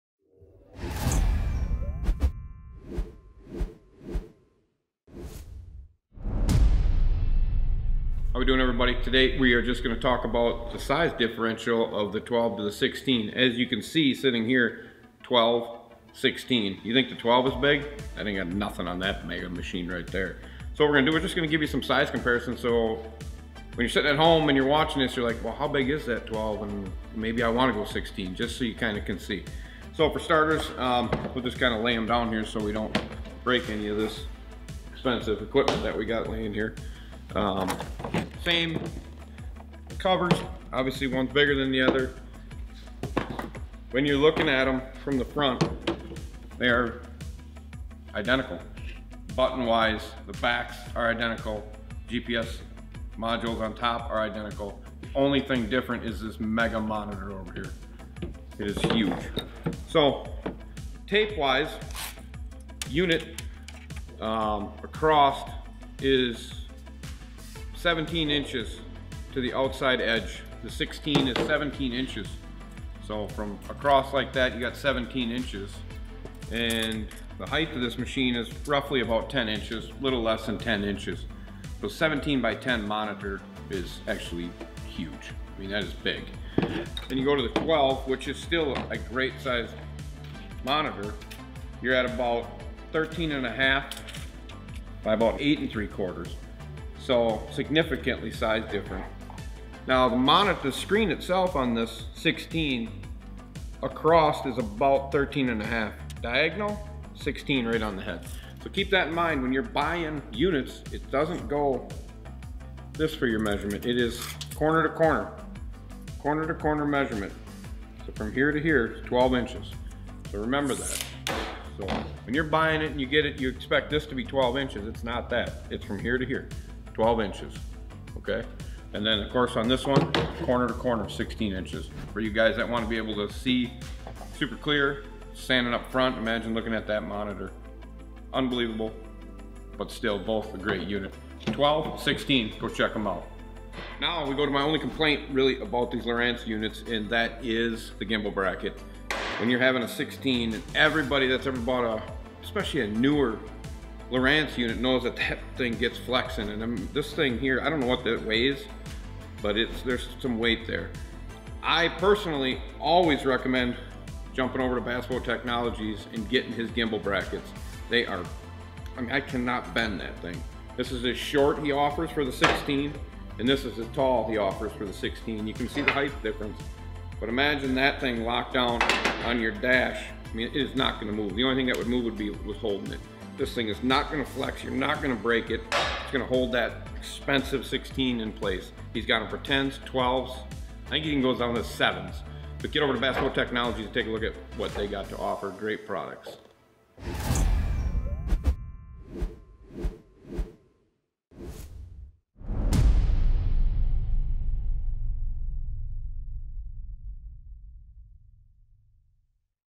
UNBOXING: Lowrance HDS 12 vs HDS 16
Ever wondered the overall size difference between a Lowrance HDS 12 versus an HDS 16? Watch Andy give ya the run down.

We are Team Marine!